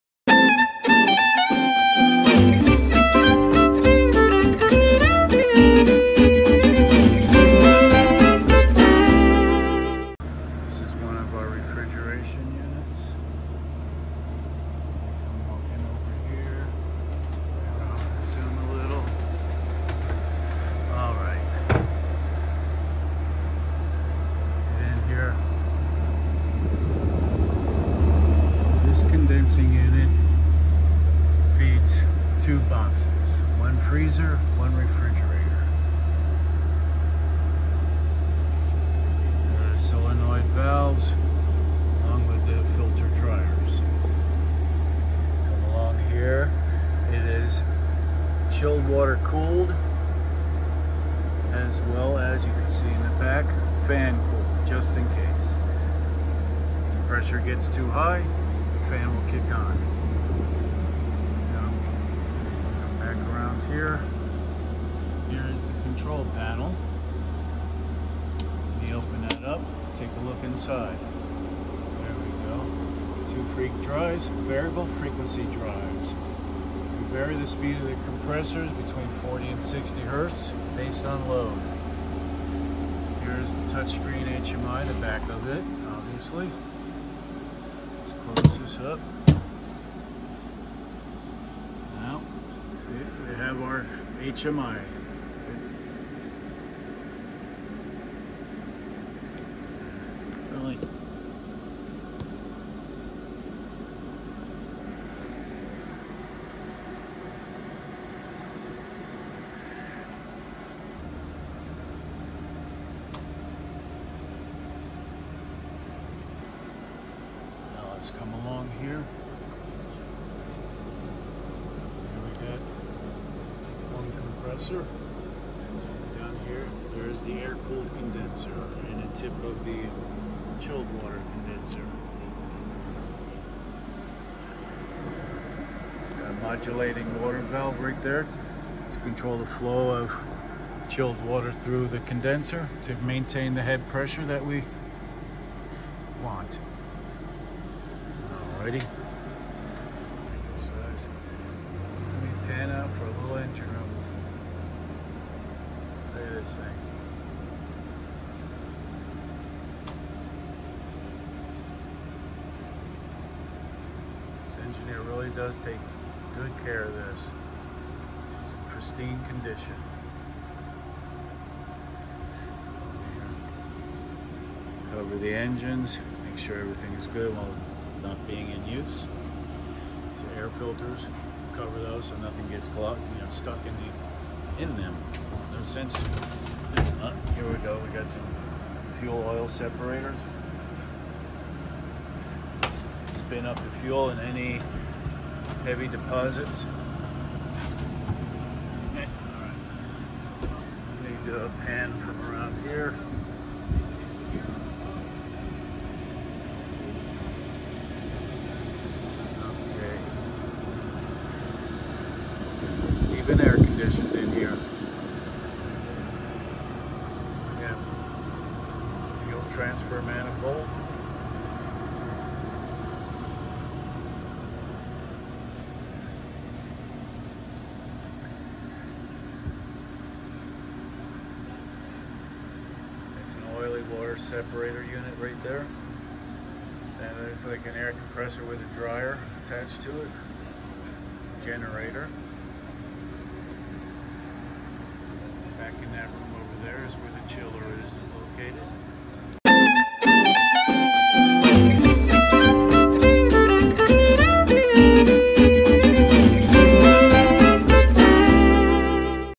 Custom Marine Refrigeration System
We designed, built and installed this refrigeration system to handle one walk-in freezer and one walk-in refrigerator onboard a 164' motor yacht.